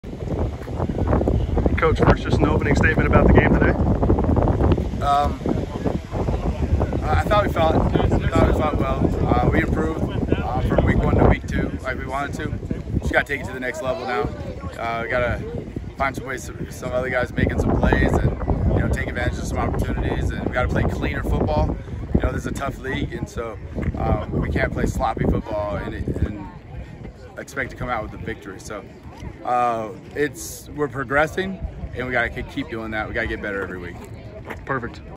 Football: Fort Lewis at New Mexico Highlands - Postgame Interview with FLC Head Coach Johnny Cox
Fort Lewis Head Coach Johnny Cox shares his thoughts after a hard fought 27-3 loss on the road at New Mexico Highlands University.